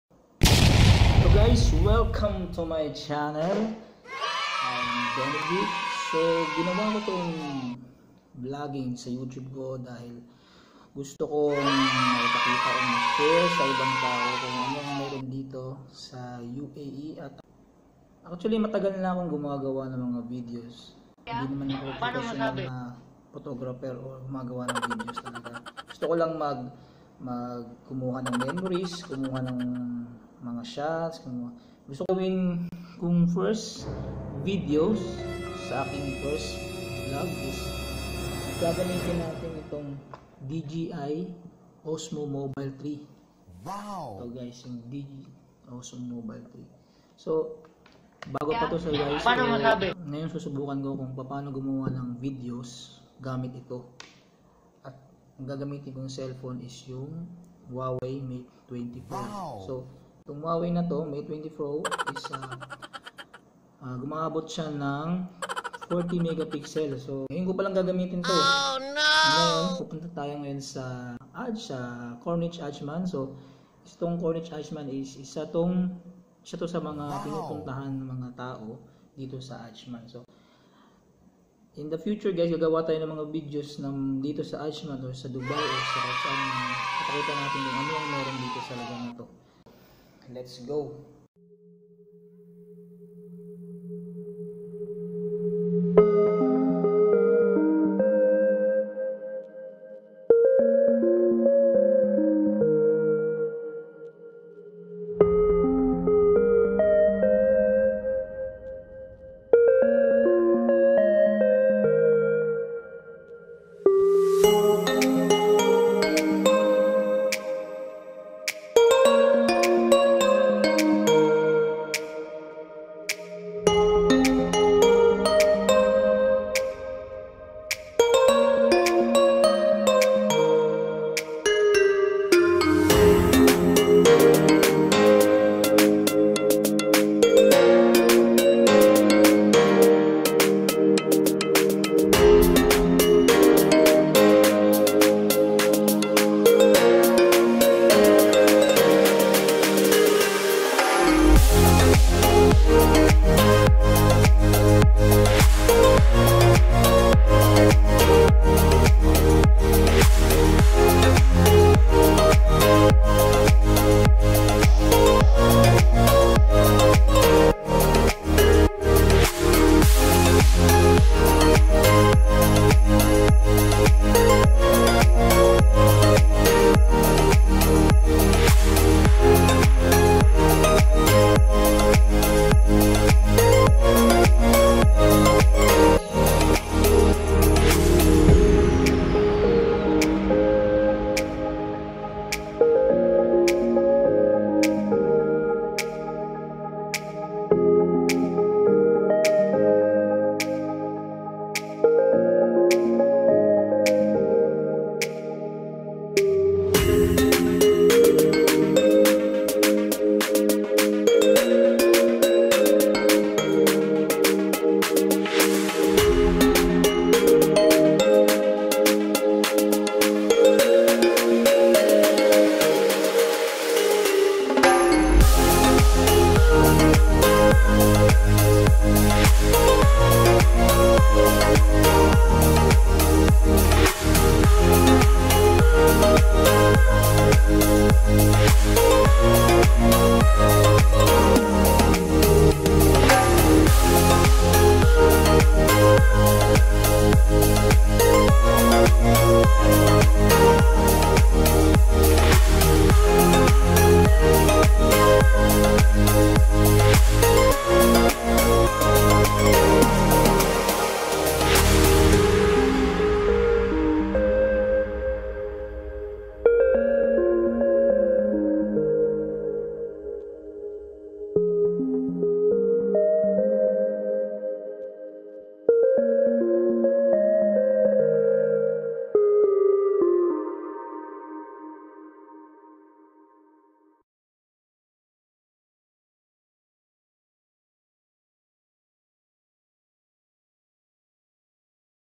How to shoot using Dji Osmo Mobile 3 at Ajman UAE- vlog01
Beautiful beach and beautiful sunset in one of the 7 City in United Arab Emirates- the Ajman.